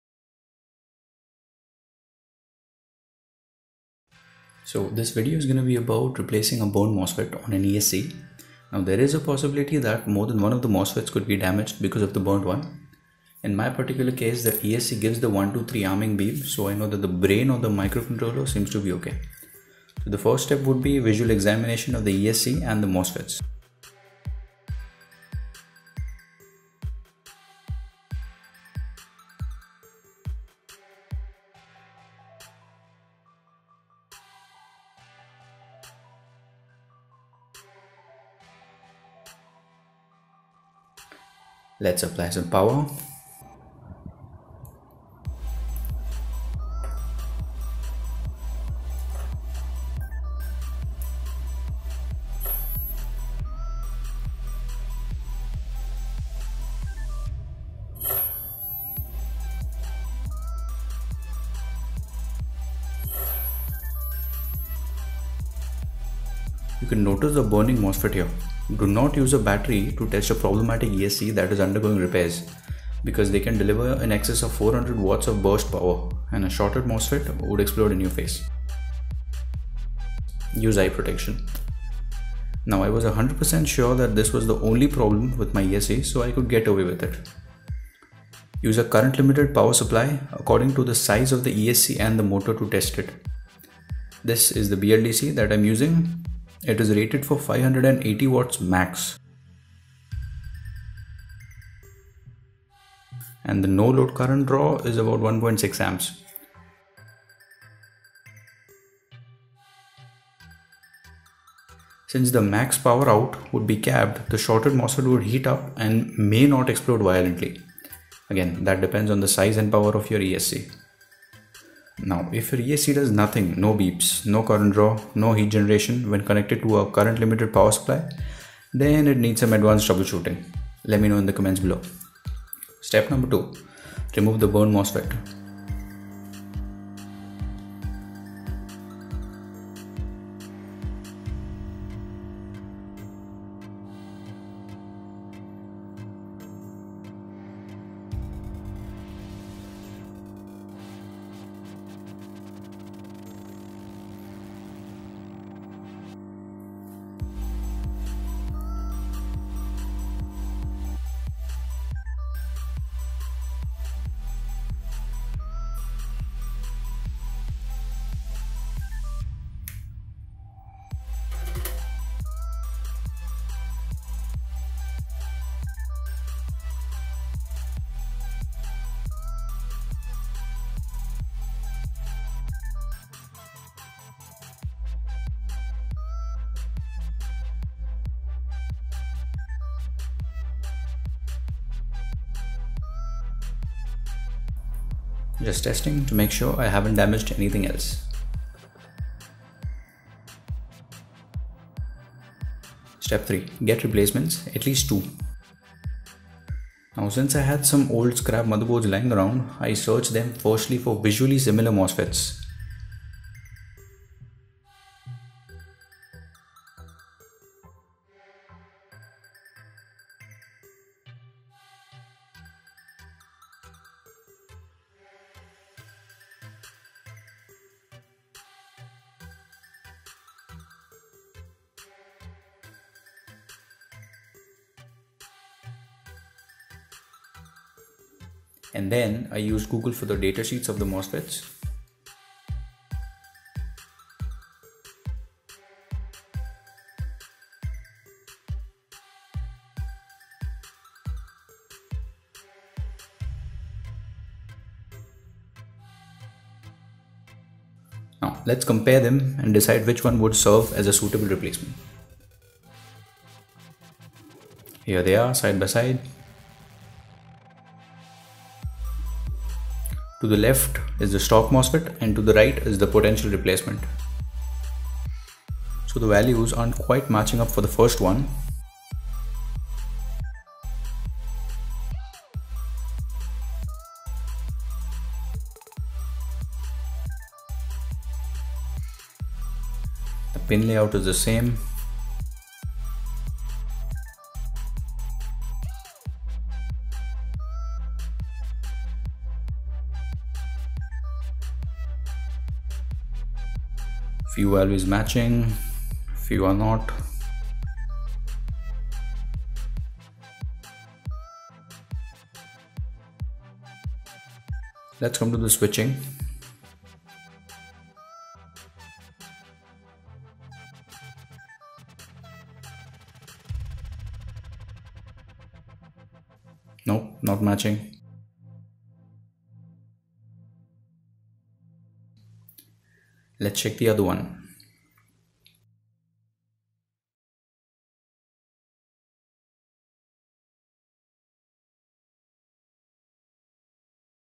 Esc Burning Mosfet Replacement Part (1/2)|DIY
(diy repair) This is the part 1 video in which I will repair an esc that has a burning/burnt mosfet on it
This is generic 30Amp chinese esc something like this

these type of escs are used in drones or diy quadcopters, rc cars etc

I found a replacement for the burnt mosfet on my esc from a scrap motherboard that i checked for compatibility with the help of the datasheet and then soldered it with a hotair rework station and it worked, do check out the second part